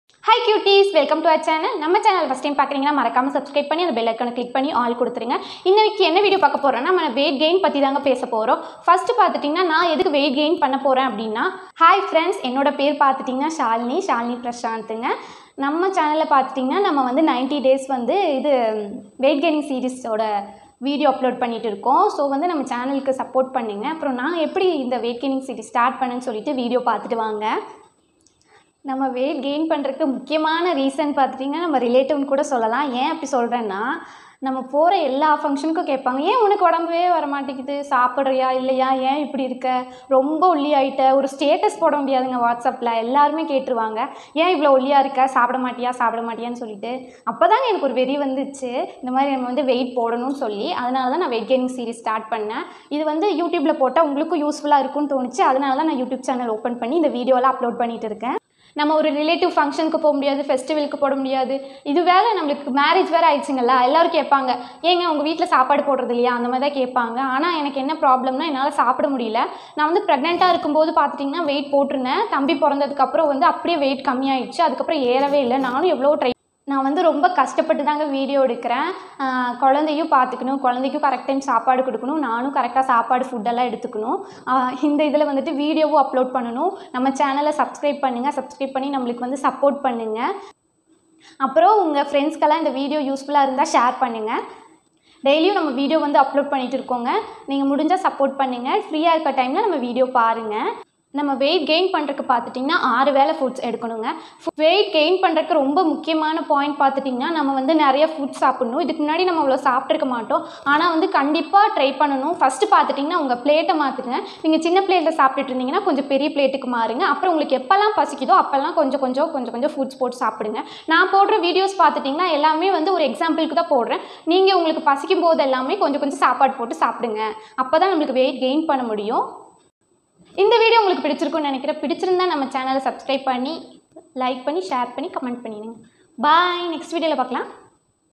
My weight gain story | Start your weight gain 90 days of challenge.
❤️Hai cutie's Welcome To "ShaluPrasanth" channel.

❤️My channel is all about women care, giving information like your family members.

Weight gain series of 90 days gives an idea about how you can increase your weight naturally & home made foods no packed foods are included in this video & it is not professional advice we are not certified 🙏.